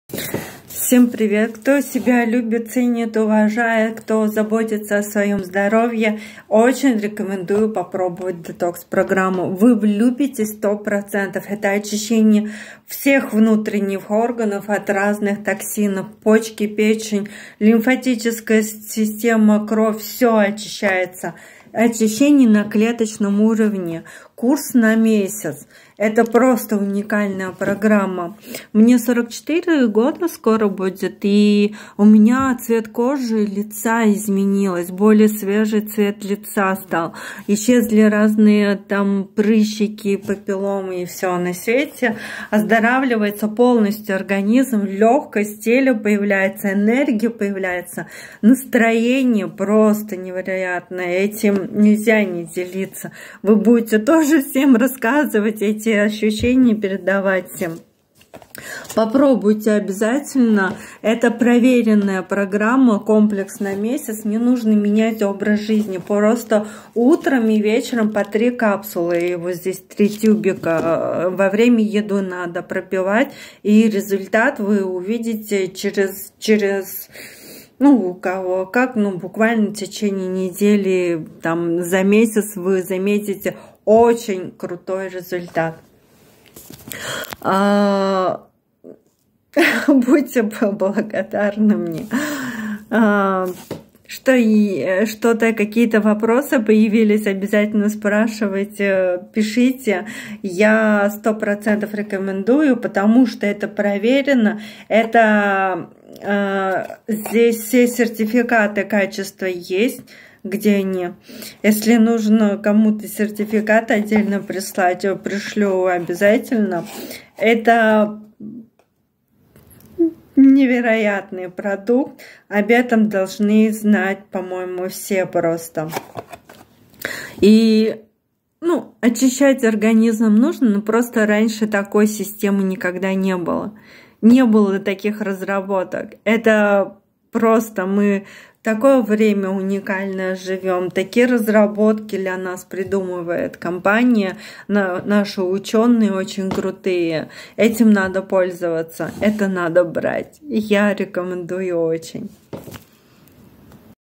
Оздоровление организма, очищение на клеточном уровне
Уникальная разработка ученых , избавление организма на клеточном уровне от всех шлаков, токсинов ..